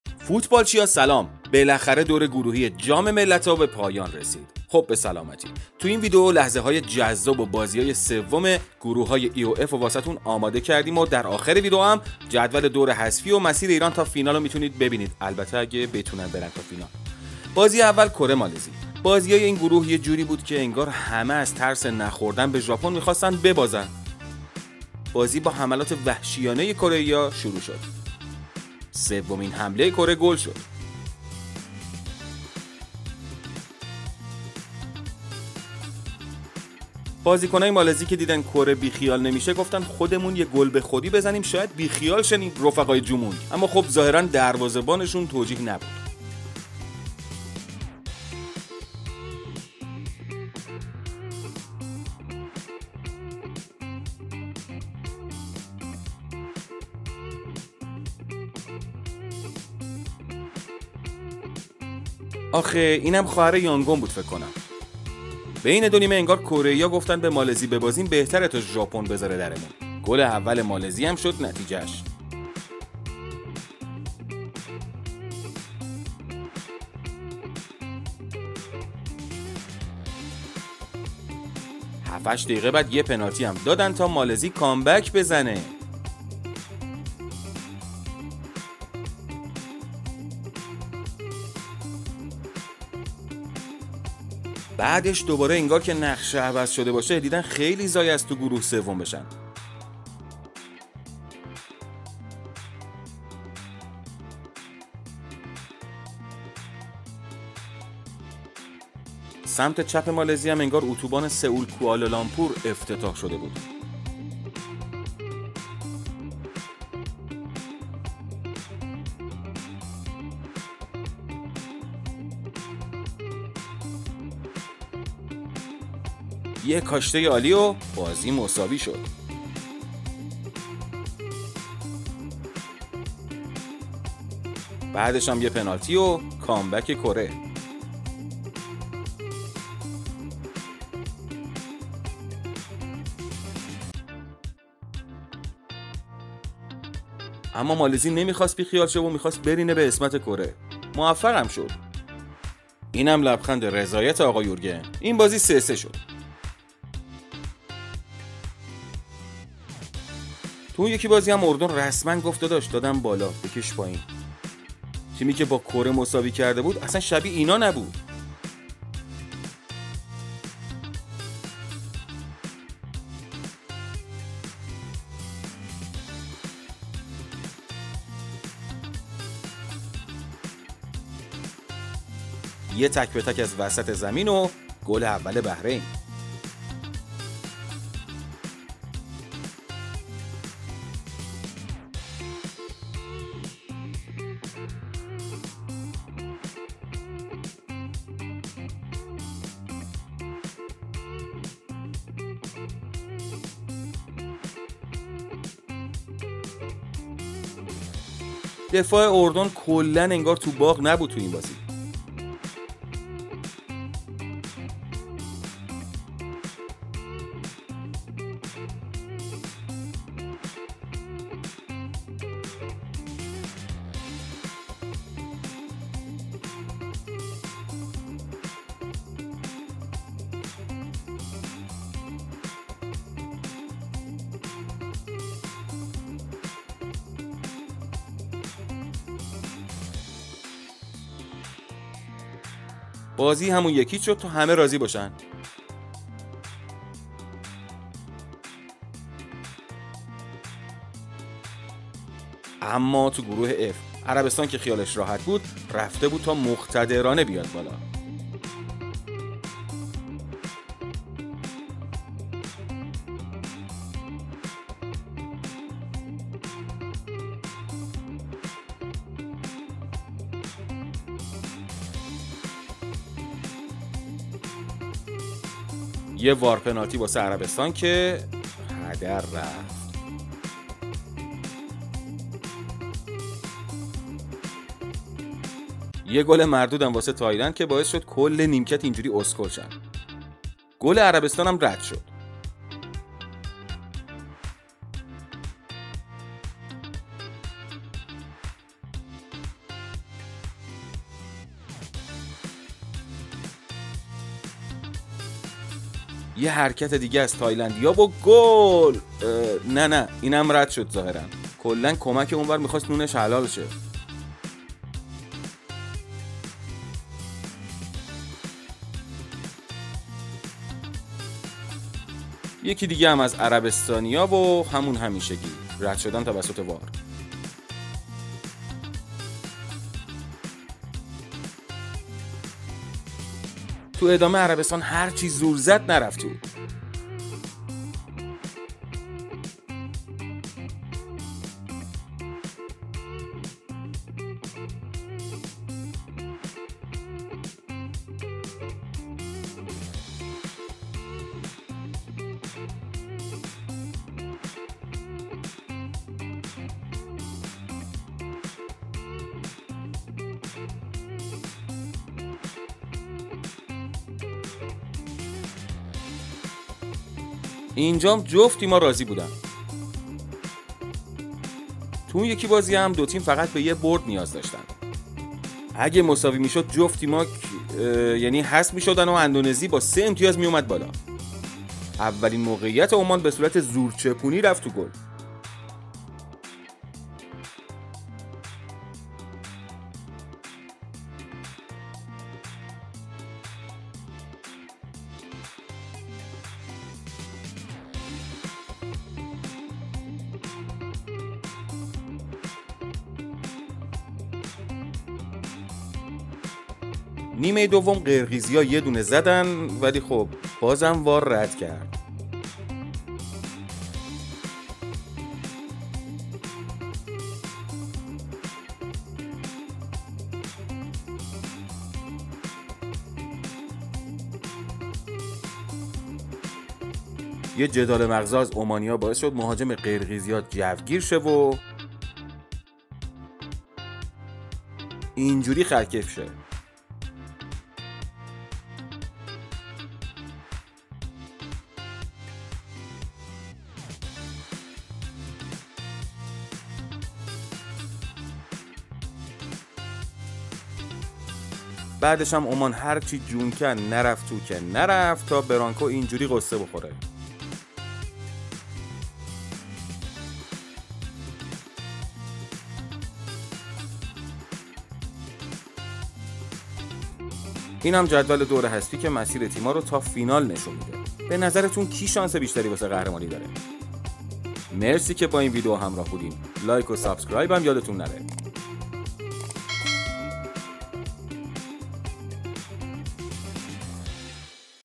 🙄 ببینید مالزی چجوری کره رو له کرد!!!
بالاخره دور گروهی جام ملتهای آسیا تموم شد و تیمای صعودکننده مشخص شدن.
مالزی که از قبل حذف شده بود بدجوری یقه ی کره رو گرفت.ظاهرا تنها تیم گروه که دوست نداشت ببازه مالزی بود چون بقیه از ترس برخورد با ژاپن بدشون نمیومد ببازن...
در نهایت همه چیز به نفع تیم سوم گروه یعنی اردن شد.تو گروه F هم زور عربستان و تایلند به هم نرسید و مساوی عمان و قرقیزستانم باعت شد تا اندونزی واسه اولین بار و فقط با یه برد صعود کنه...

جدول دور حذفی و مسیر تیما تا فینال و میتونید آخر این ویدئو ببینید

به نظرتون کدوم تیم مسیر راحت تری واسه قهرمانی داره؟؟؟

نظرتون و واسمون کامنت کنید